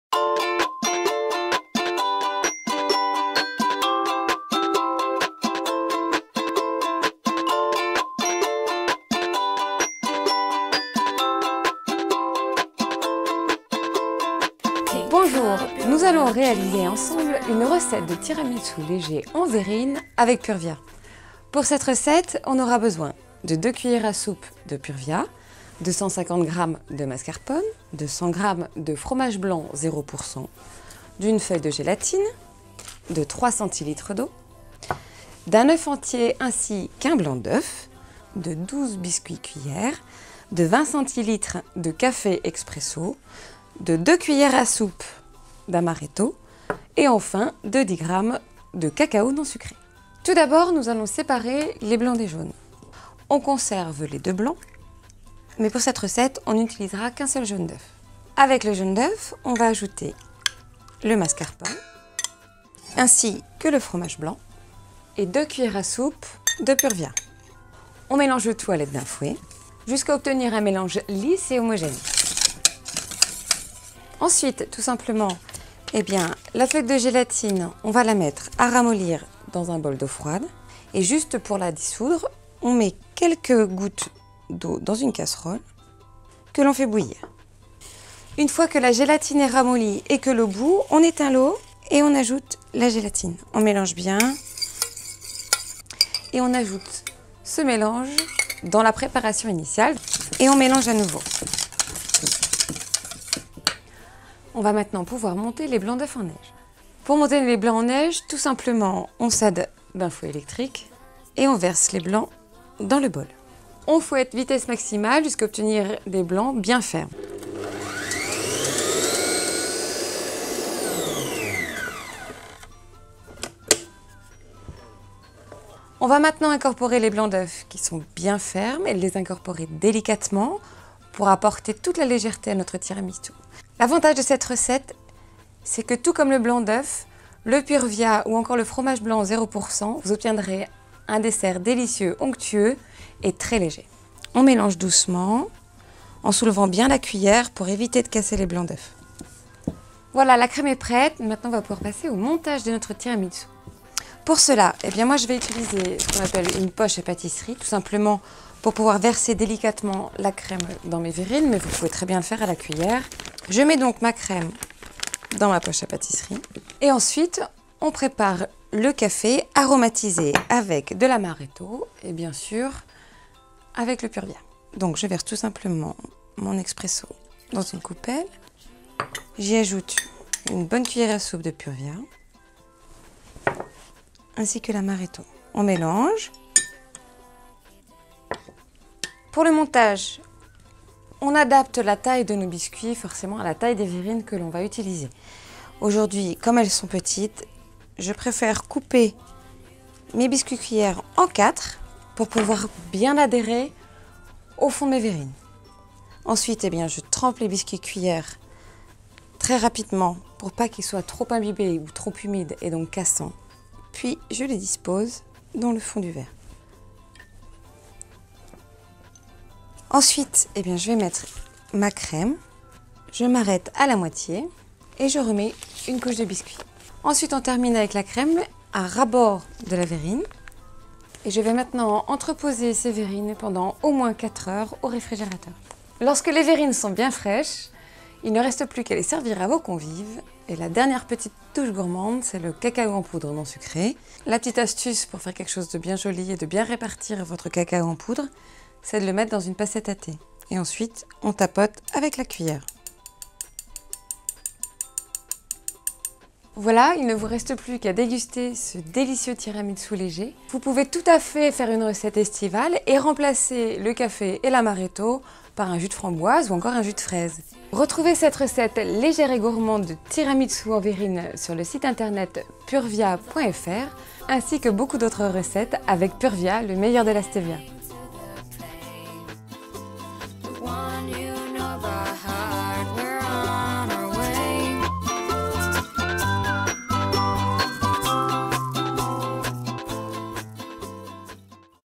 Pure Via - Recette du tiramisu allégé à l'extrait de Stévia
Le tiramisu est un dessert copieux mais simple à réalisé. Grâce à Pure Via cessez de culpabiliser et découvrez notre recette élaborée par un chef, allégée en calories. Attention, la poudre utilisée dans la recette est dorénavant remplacée par la poudre cristallisée Pure Via.